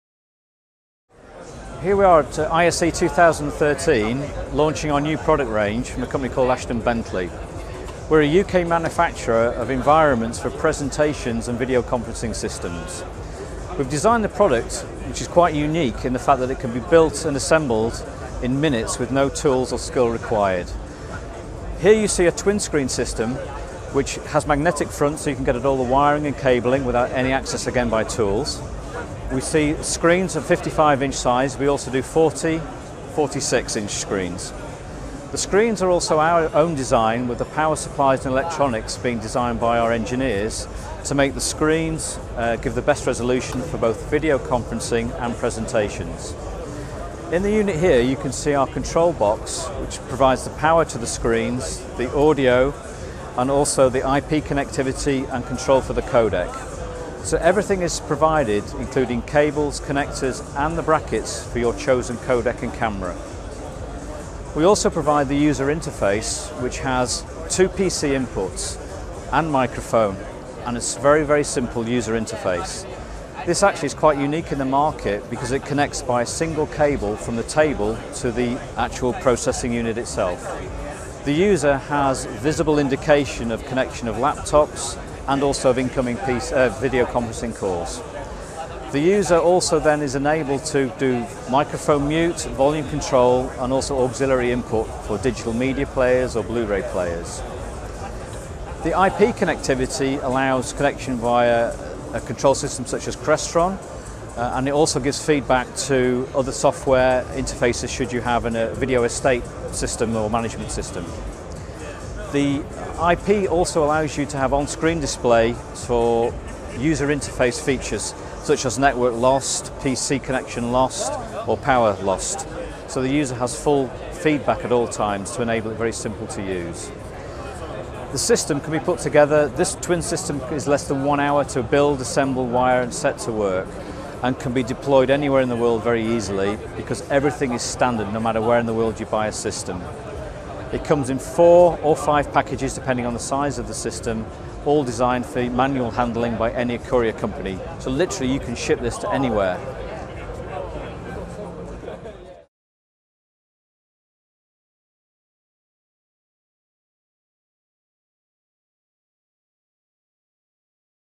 ISE 2013: Ashton Bentley - New at ISE - Walk in and work
Complete presentation and video conference environments in a box. Everything required to provide a solution- display, chassis, control, user interface and IP tools. Assembled in minutes with no skill, experience or tools! Shown at ISE 2013.